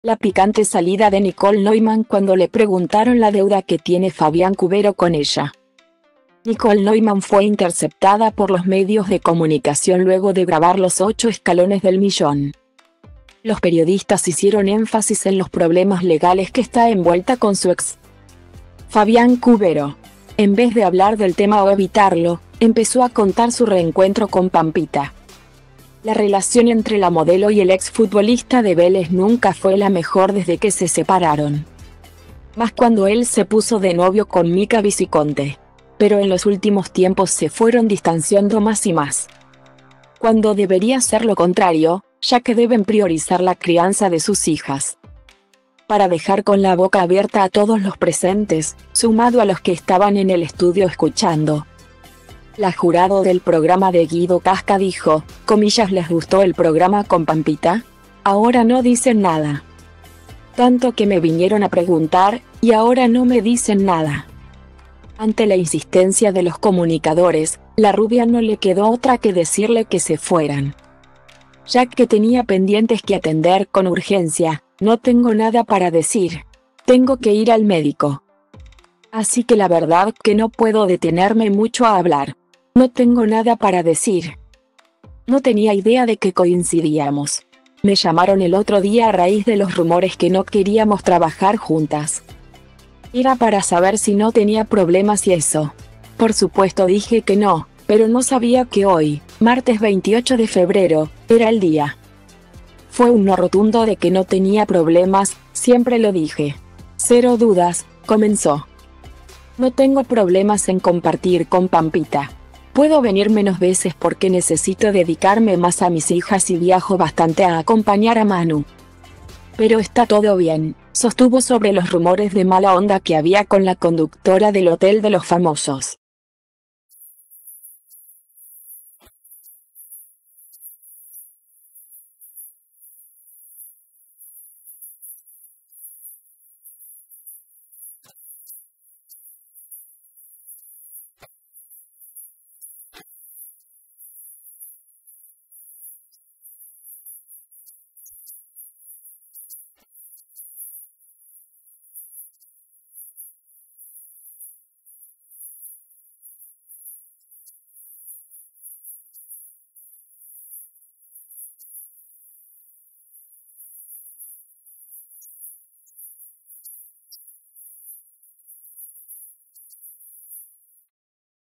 La picante salida de Nicole Neumann cuando le preguntaron la deuda que tiene Fabián Cubero con ella
Nicole Neumann fue interceptada por los medios de comunicación luego de grabar Los 8 Escalones del Millón.Los periodistas hicieron énfasis en los problemas legales que está envuelta con su ex,Fabián Cubero. En vez de hablar del tema o evitarlo, empezó a contar su reencuentro con Pampita.